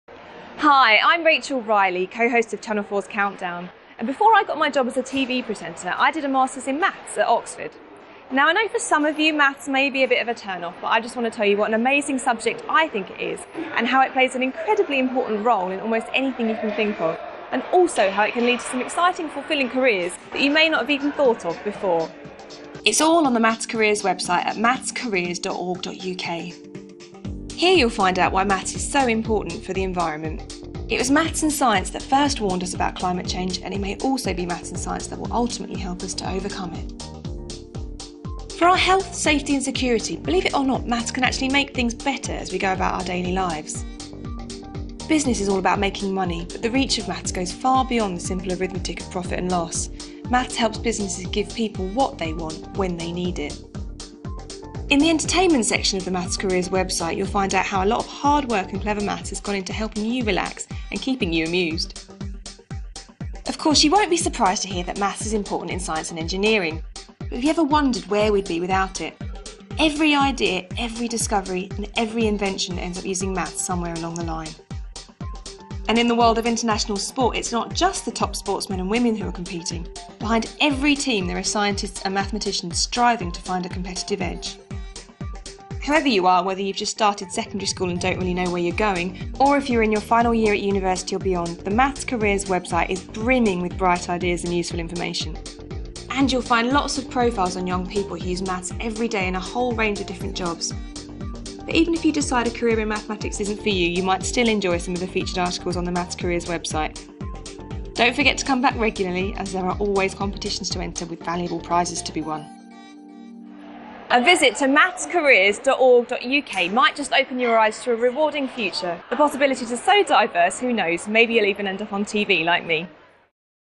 Introduction to
Presented by Rachel Riley and produced by the Institute of Mathematics & its Applications (IMA) as part of the National HE STEM Programme